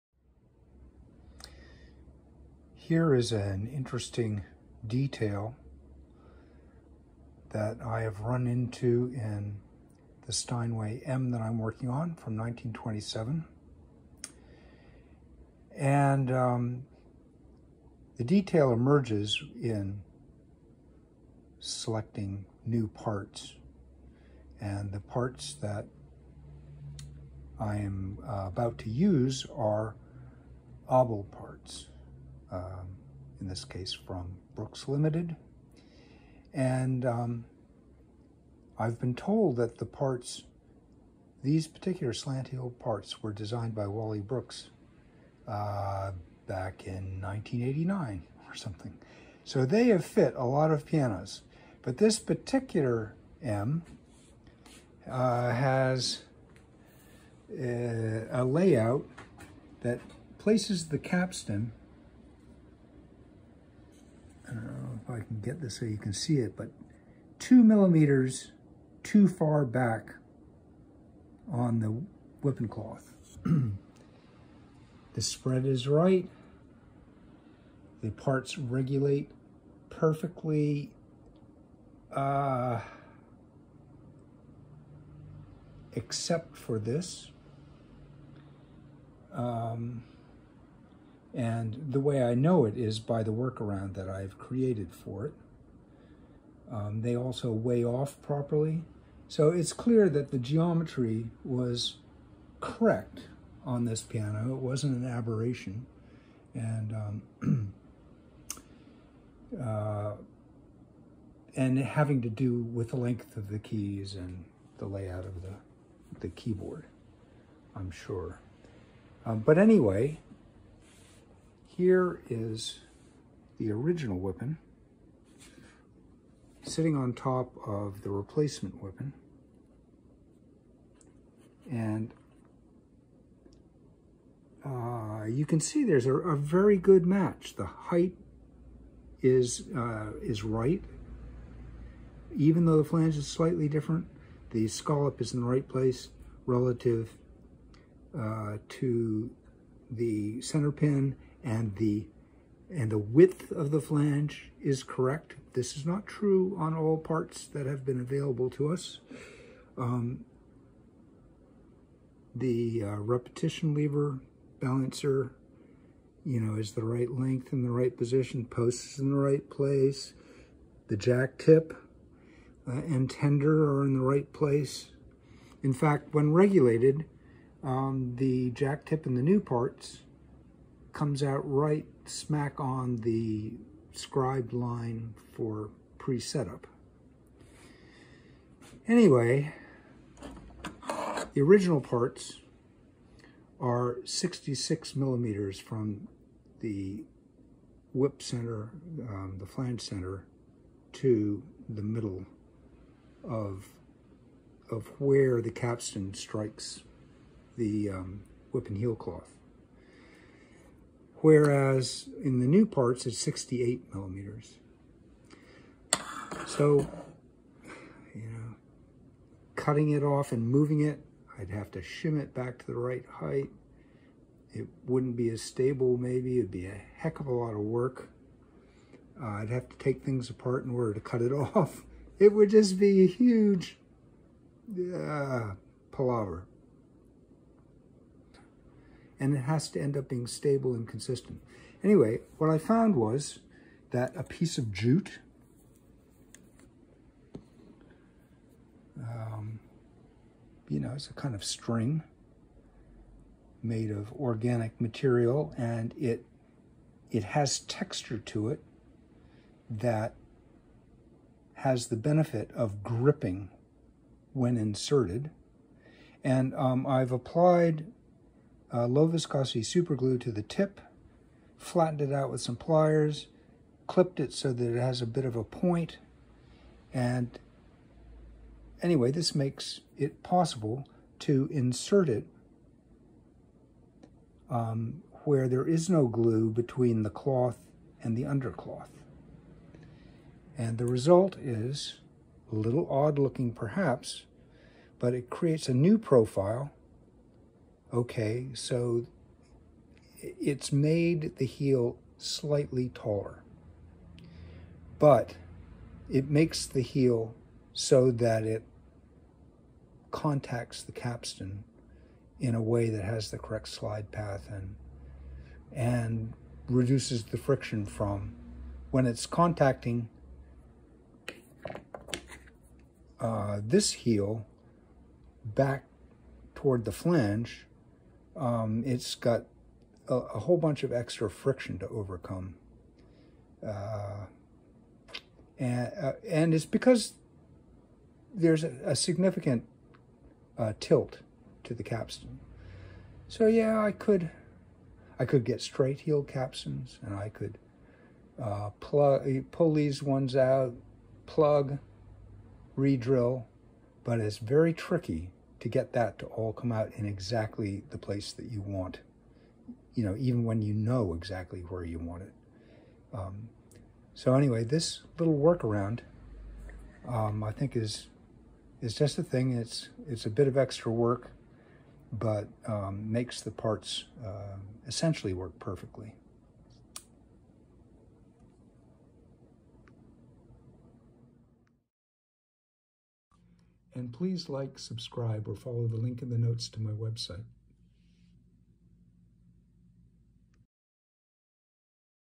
Wippen Heel Solution
When new parts for a slant heel Steinway match in every way except the heel position is 2mm off, use jute string to reposition the heel profile.